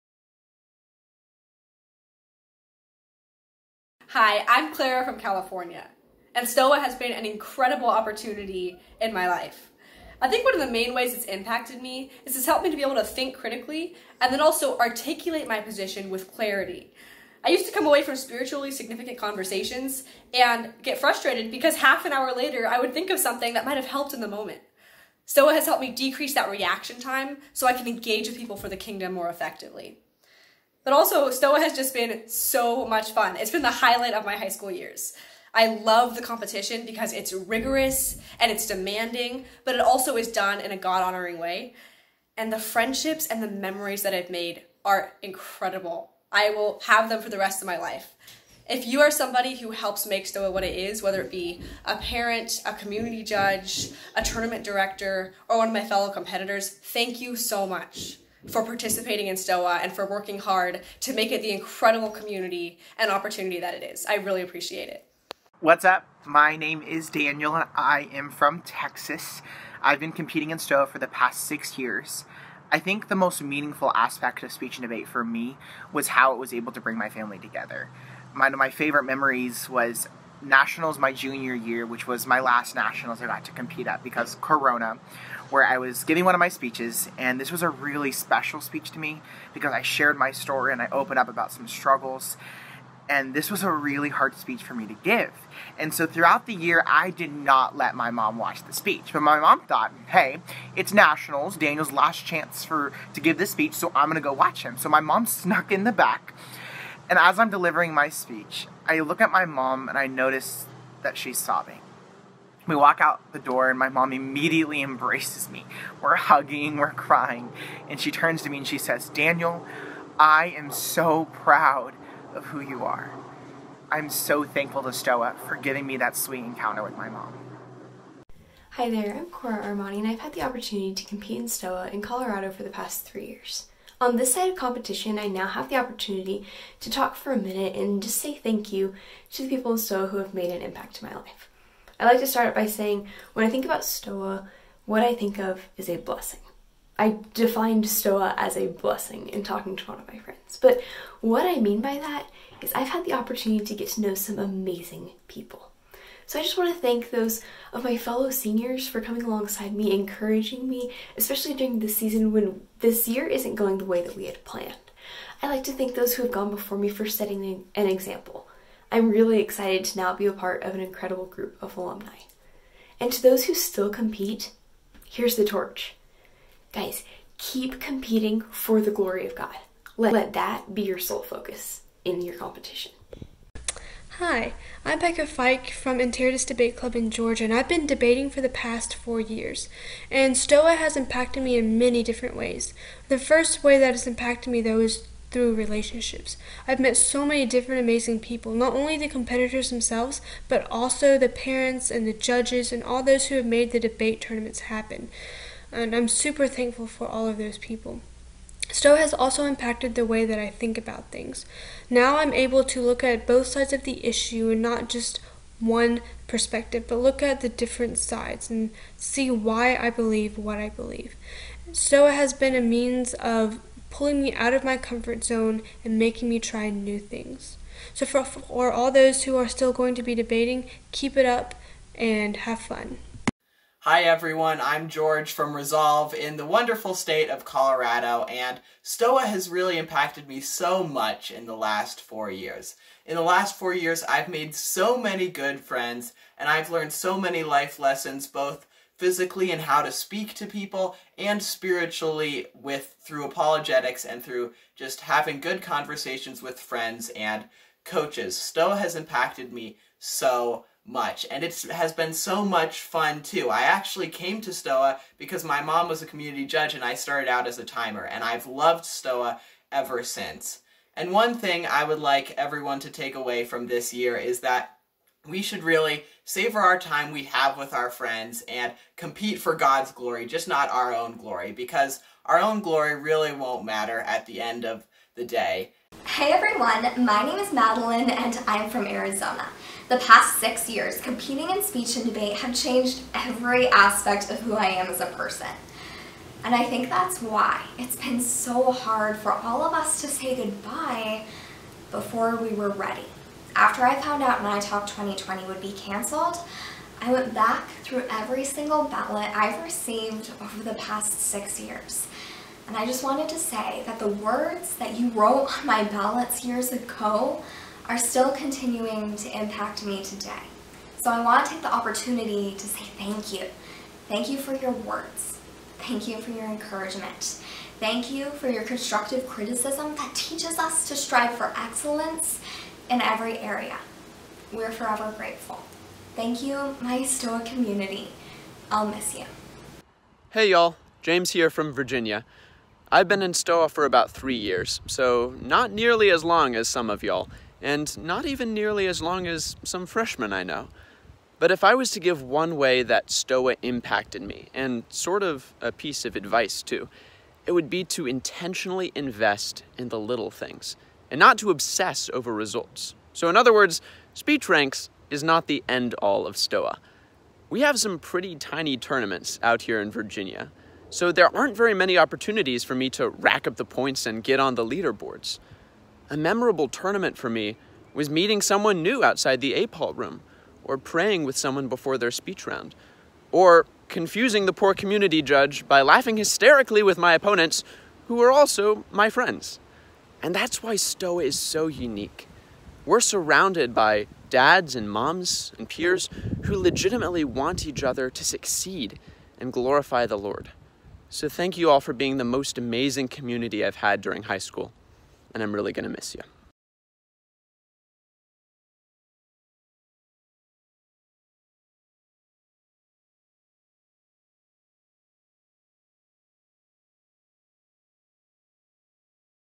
2020 Stoa Seniors Speak: #5
In our fifth release of Stoa Seniors Speak, we hear from 2020 graduates Clara, Daniel, Cora, Becca, George, Madelyn, and James as they reflect on their time in Stoa.